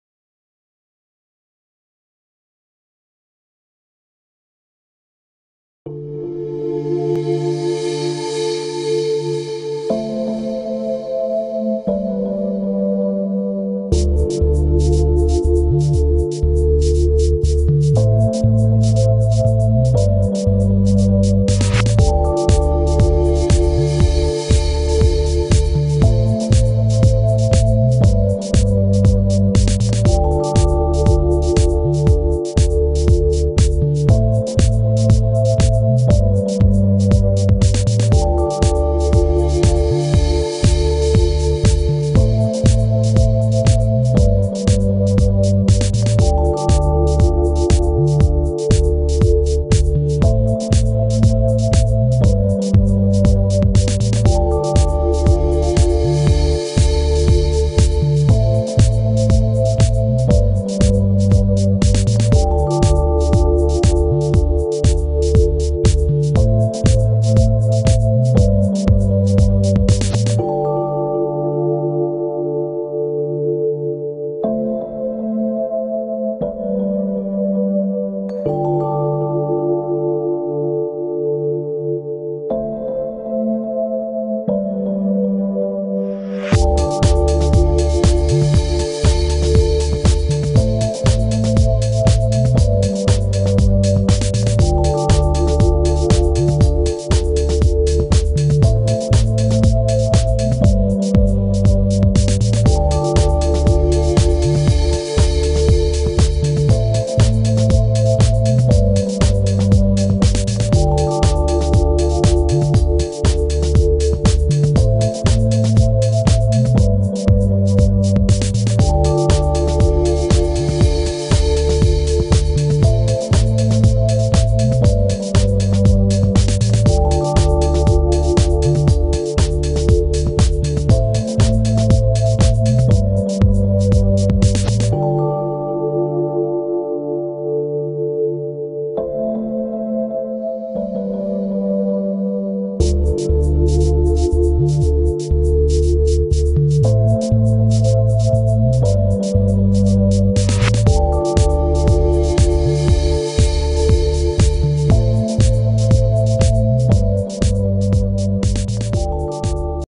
Numatic TTB1840 Scrubber Dryer
This is a video showing how to use the Numatic TTB1840 Scrubber Dryer.